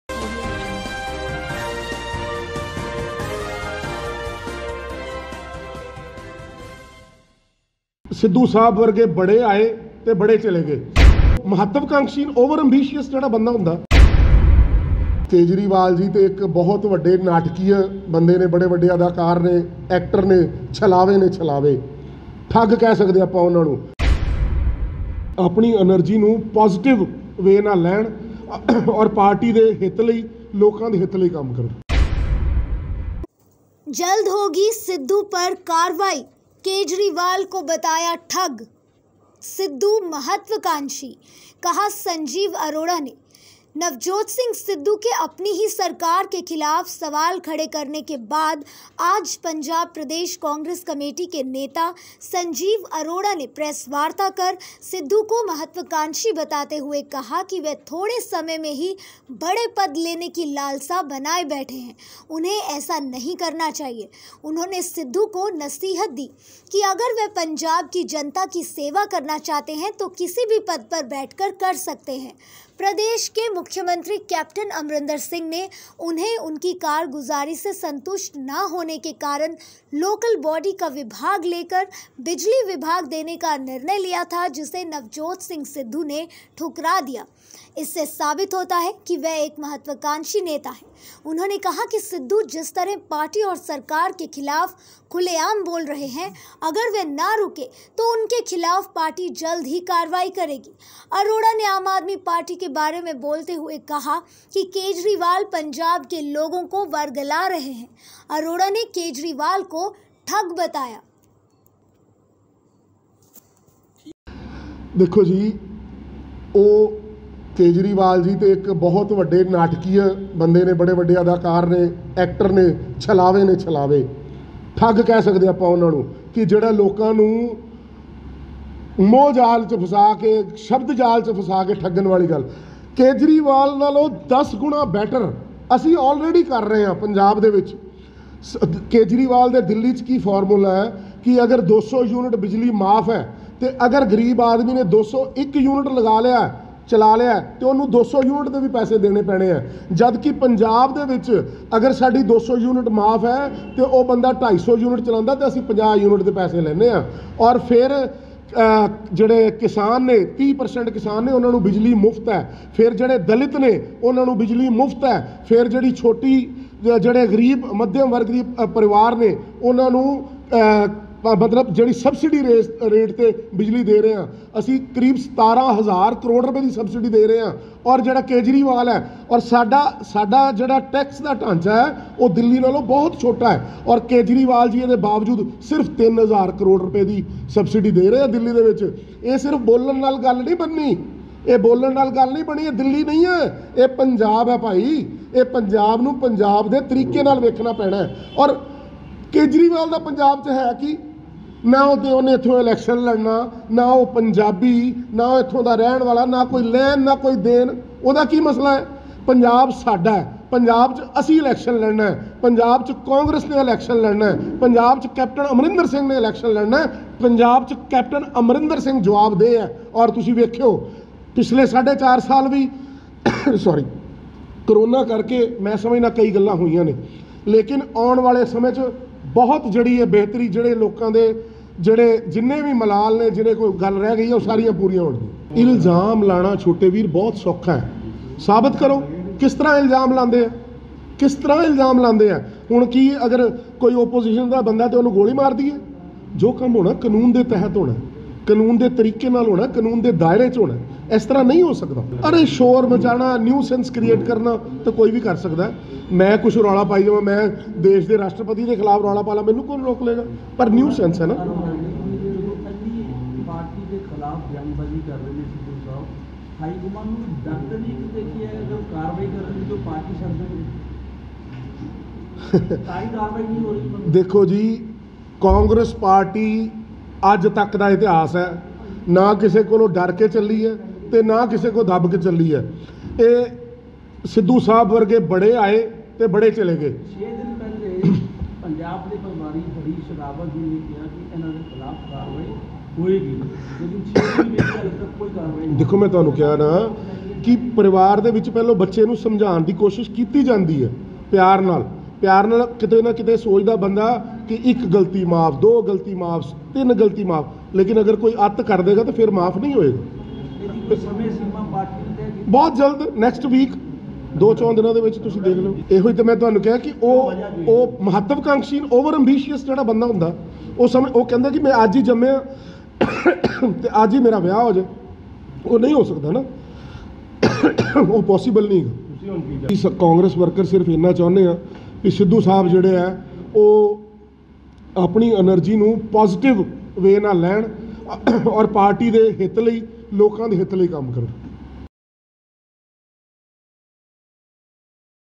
जल्द होगी सिद्धू पर कार्रवाई
केजरीवाल को बताया ठग
सिद्धू महत्वकांक्षी: संजीव अरोड़ा
नवजोत सिंह सिद्धू के अपनी ही सरकार के खिलाफ सवाल खड़े करने के बाद आज पंजाब प्रदेश कांग्रेस कमेटी के नेता संजीव अरोड़ा ने प्रेस वार्ता कर सिद्धू को महत्वकांक्षी बताते हुए कहा कि वह थोड़े समय में ही बड़े पद लेने की लालसा बनाए बैठे हैं. उन्हें ऐसा नहीं करना चाहिए. उन्होंने सिद्धू को नसीहत दी कि अगर वह पंजाब की जनता की सेवा करना चाहते हैं तो किसी भी पद पर बैठकर कर सकते हैं. प्रदेश के मुख्यमंत्री कैप्टन अमरिंदर सिंह ने उन्हें उनकी कारगुजारी से संतुष्ट ना होने के कारण लोकल बॉडी का विभाग लेकर बिजली विभाग देने का निर्णय लिया था जिसे नवजोत सिंह सिद्धू ने ठुकरा दिया. इससे साबित होता है कि वह एक महत्वकांक्षी नेता है. उन्होंने कहा कि सिद्धू जिस तरह पार्टी और सरकार के खिलाफ खुलेआम बोल रहे हैं अगर वह ना रुके तो उनके खिलाफ पार्टी जल्द ही कार्रवाई करेगी. अरोड़ा ने आम आदमी पार्टी के बारे में बोलते हुए कहा कि केजरीवाल पंजाब के लोगों को वरगला रहे हैं. अरोड़ा ने केजरीवाल को ठग बताया.

15 years old Public Dilasa Newspaper and Public Dilasa Channel is very popular source of Amritsar's latest news and is very active on Facebook, Youtube, Instagram and Twitter.